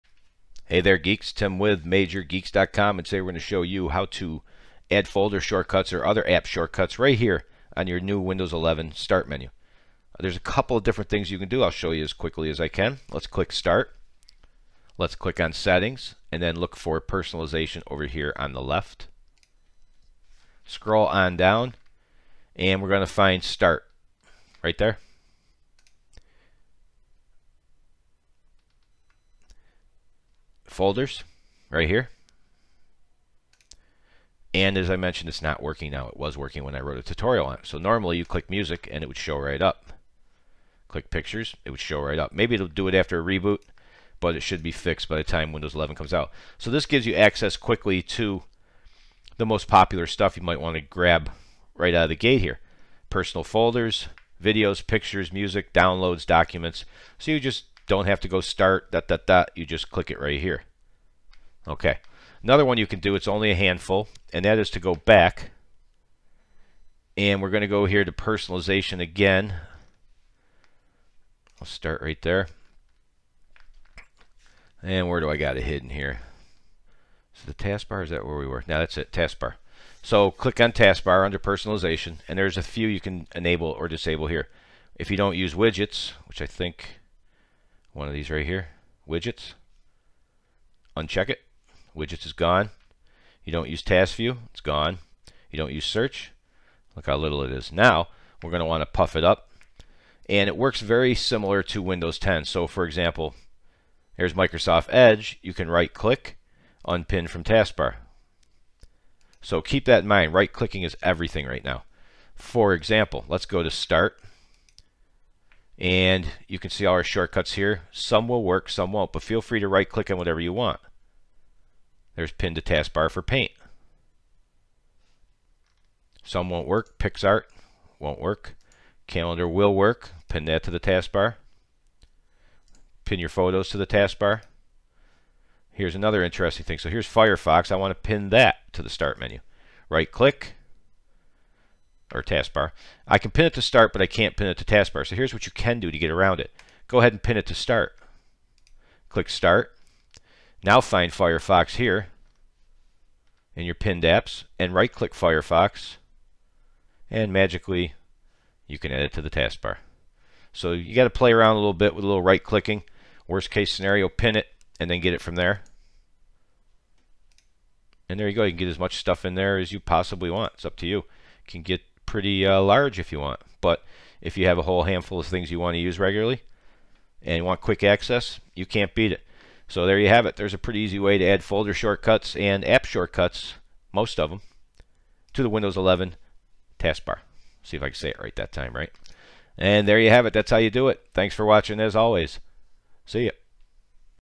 How to Add Folder and App Shortcuts to The Windows 11 Start Menu
The Windows 11 Start Menu is different than before and takes some getting used to. One new feature is adding folders or apps directly to the Start Menu to access the most commonly used folders easily.